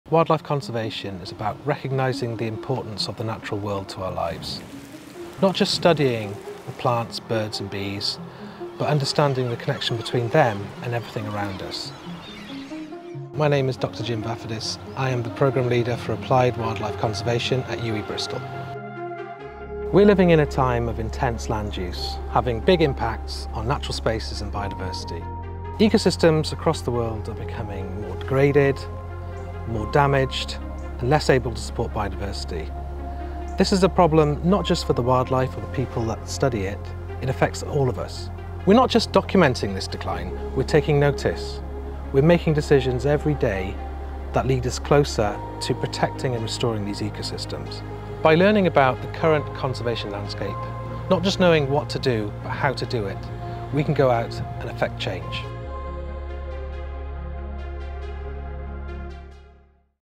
Choose MSc Applied Wildlife Conservation for Postgraduate Study
Explore the importance of the natural world and how understanding our current conservation landscape helps us to protect ecosystems and biodiversity.